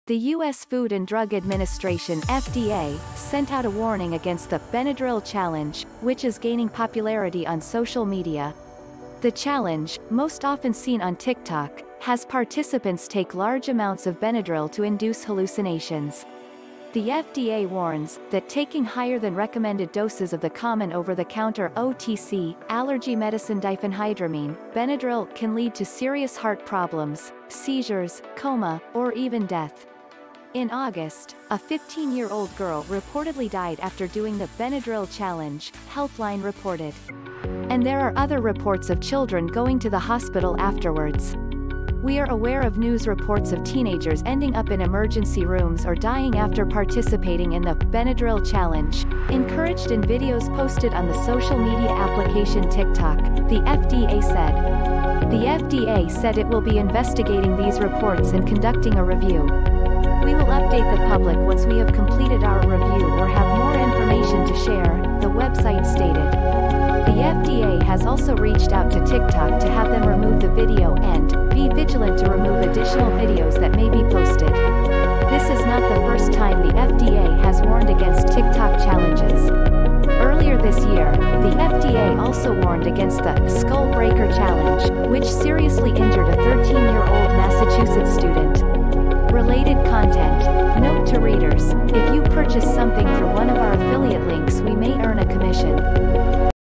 FDA warns against ’Benadryl Challenge’ promoted on TikTok
The U. S. Food and Drug Administration (FDA) sent out a warning against the “Benadryl Challenge”, which is gaining popularity on social media. The challenge, most often seen on TikTok, has participants take large amounts of Benadryl to induce hallucinations. The FDA warns “that taking higher than recommended doses of the common over-the-counter (OTC) allergy medicine diphenhydramine (Benadryl) can lead to serious heart problems, seizures, coma, or even death.”In August, a 15-year-old girl reportedly died after doing the “Benadryl challenge,'' Healthline reported. And there are other reports of children going to the hospital afterwards.“We are aware of news reports of teenagers ending up in emergency rooms or dying after participating in the ‘Benadryl Challenge’ encouraged in videos posted on the social media application TikTok,” the FDA said. The FDA said it will be investigating these reports and conducting a review.“We will update the public once we have completed our review or have more information to share,” the website stated. The FDA has also reached out to TikTok to have them remove the video and “be vigilant to remove additional videos that may be posted.”This is not the first time the FDA has warned against TikTok challenges. Earlier this year, the FDA also warned against the “Skull Breaker Challenge,'' which seriously injured a 13-year-old Massachusetts student.